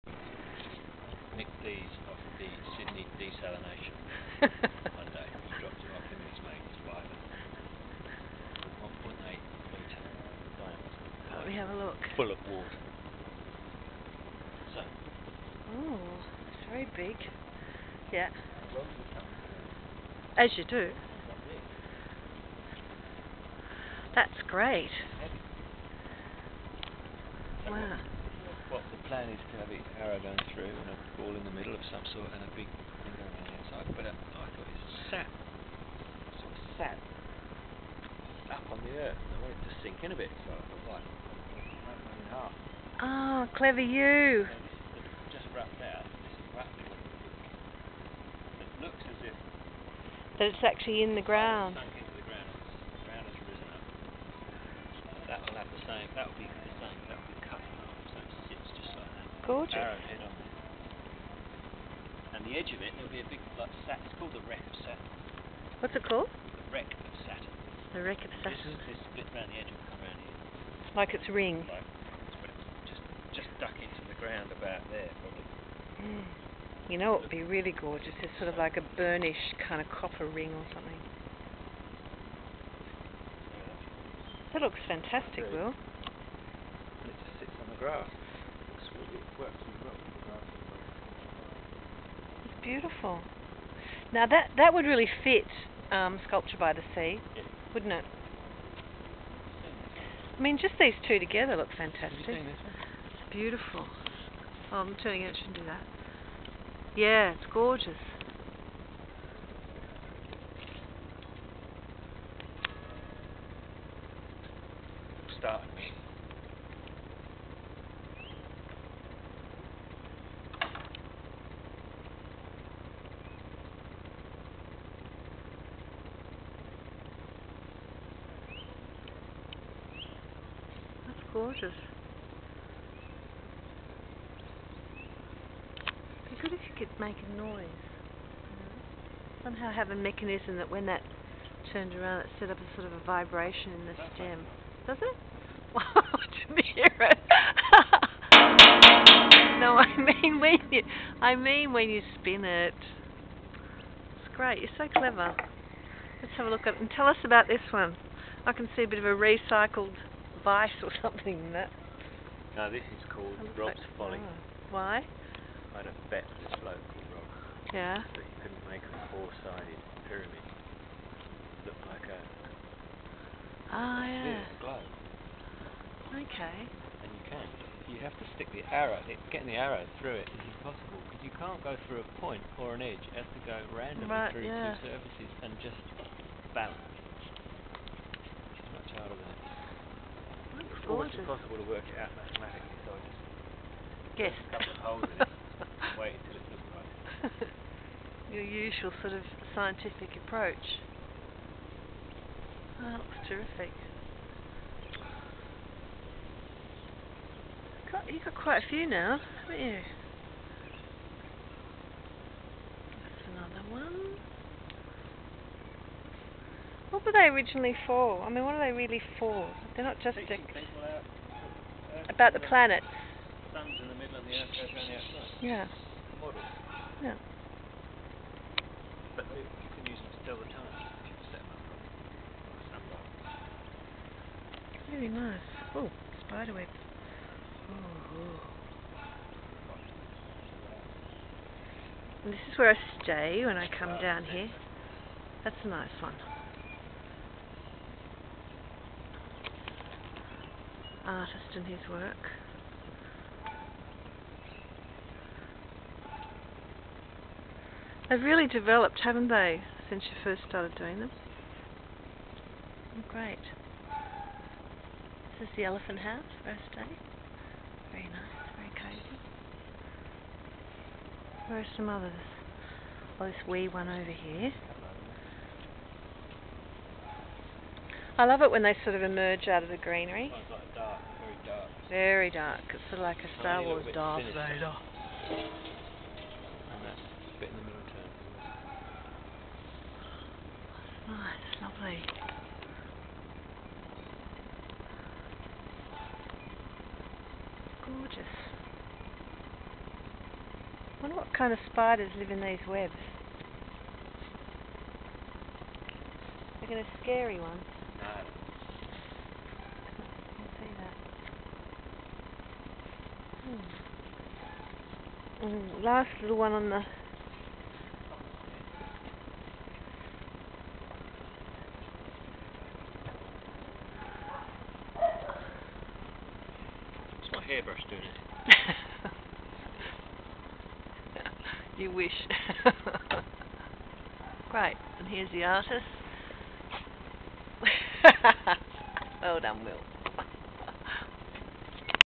Will and his armillary spheres -Sydney, April 2010.MOV
Sydney -based Will Upjohn, designs and makes armillary spheres ( a spherical model of the universe, first used by early Greek astronomers, in which the relative positions of the Earth and other astronomical objects are represented by intersecting metal rings - MSN Encarta dictionary)
 They're beautiful!